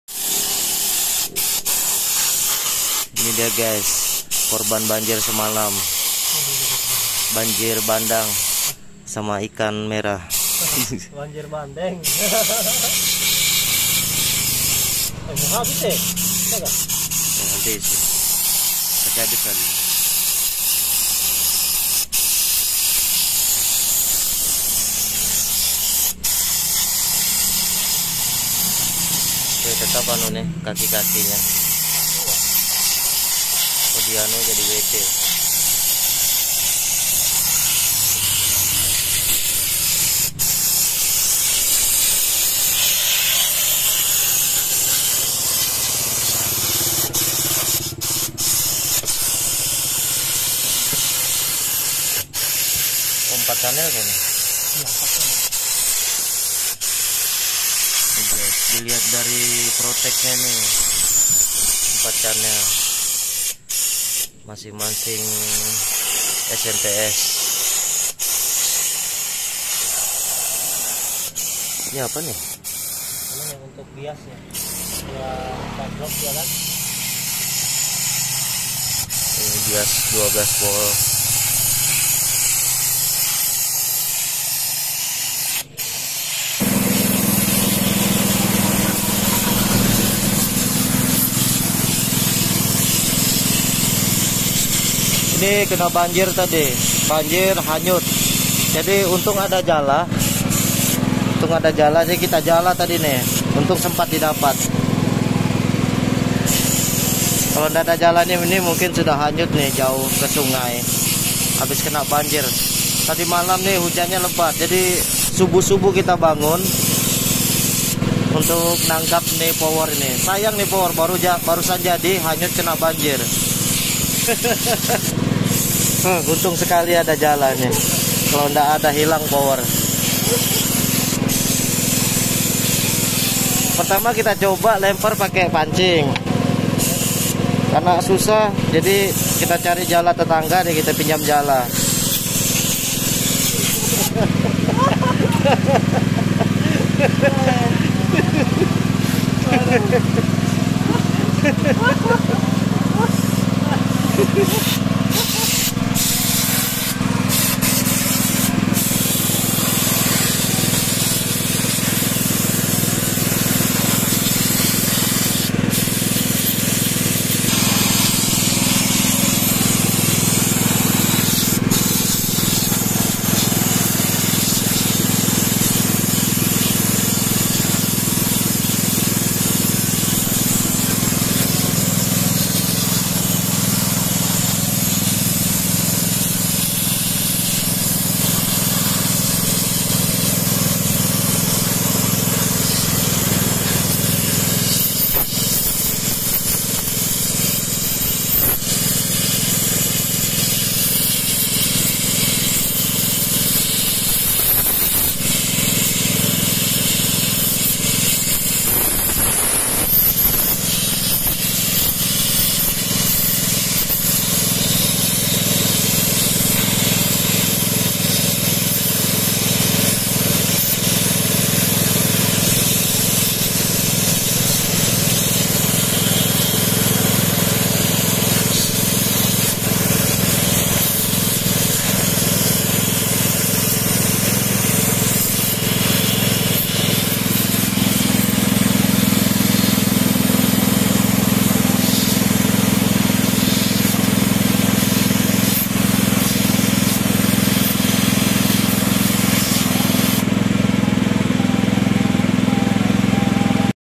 ketika power klas D, terkena banjir
tadimalam hujan deras, sehingga air meluap
dan mengakibatkan banjir masuk kerumah dan merendam beberapa alat elektronik dam power